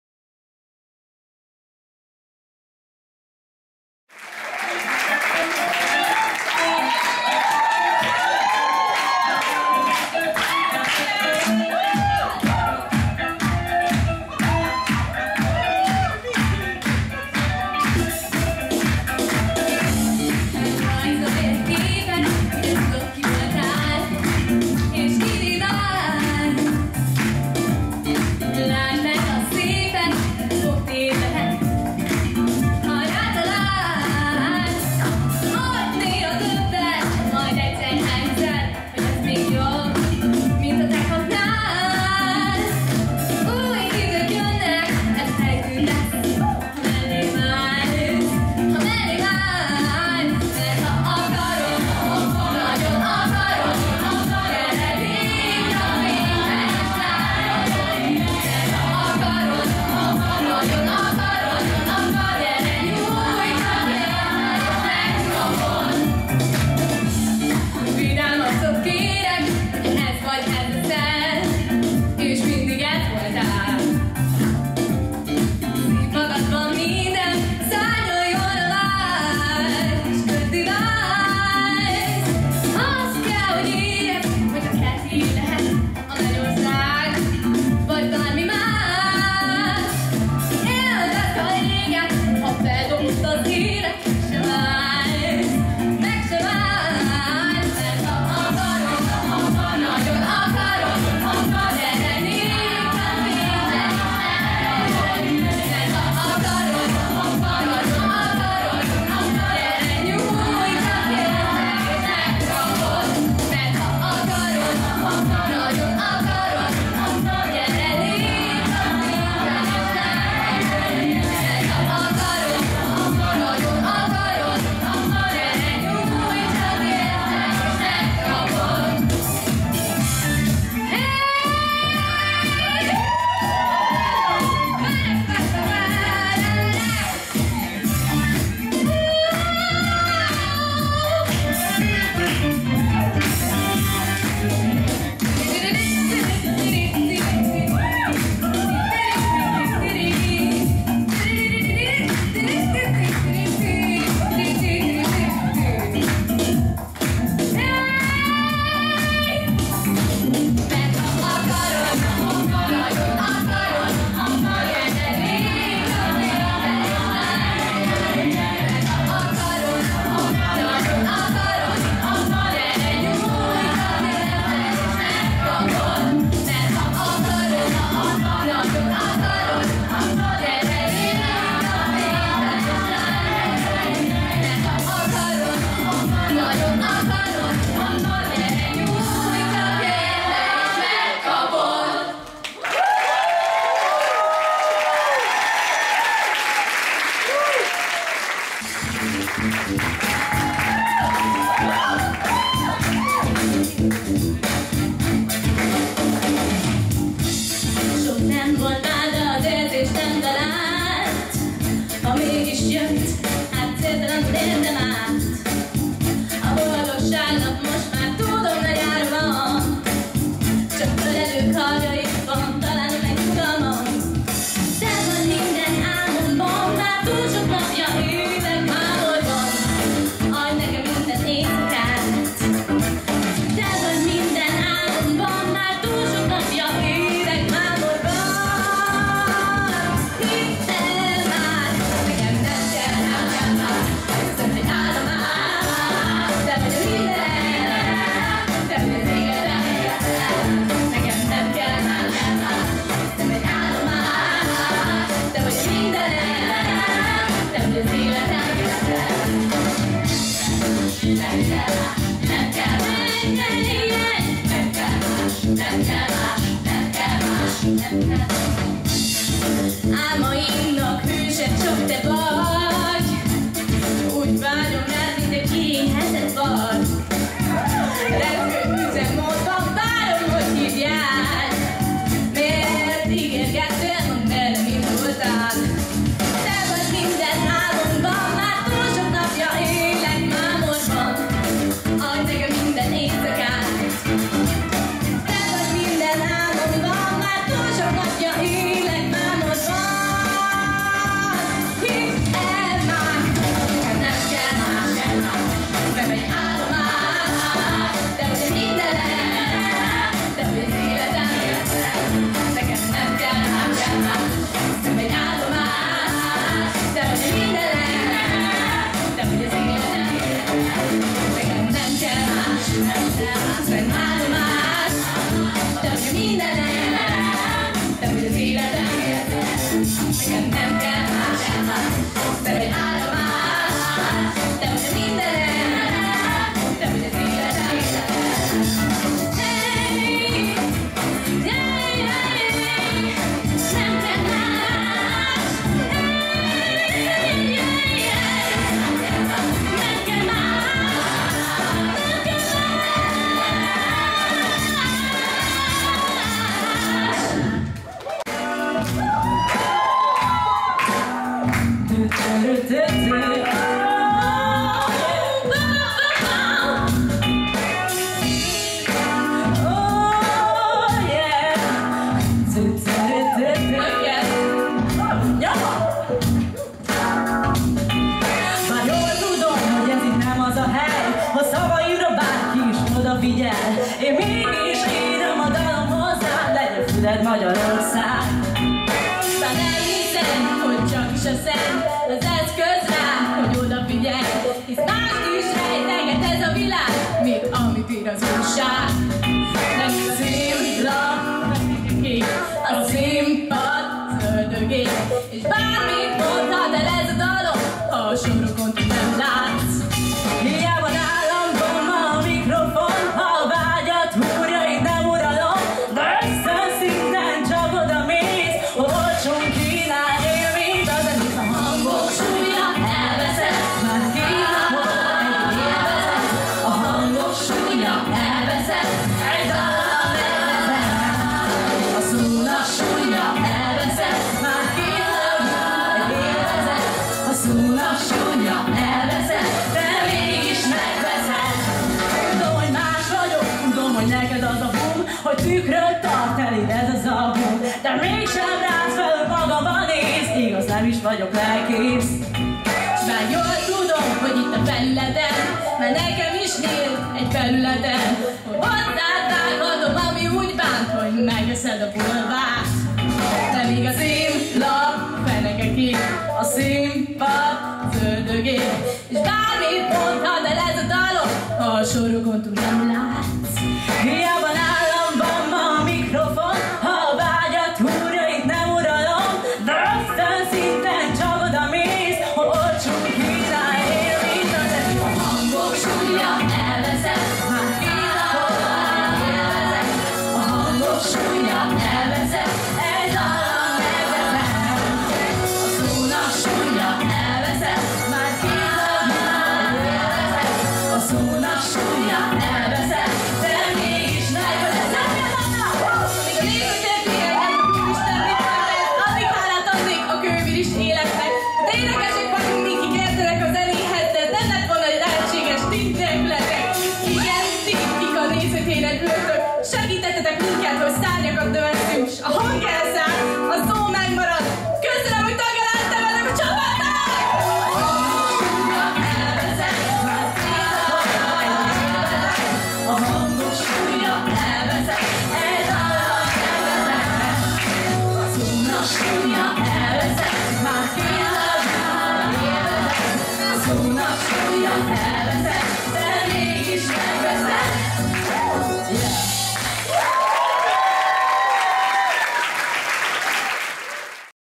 Kerekes Lányok és a United dalai a Kővirágok Showkórus vizsgáján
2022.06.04-én a Kővirágok zeneiskola Fészek klubban rendezett fellépése alkalmával a Kerekes lányok és Mezey Diána előadásában elhangzott dalok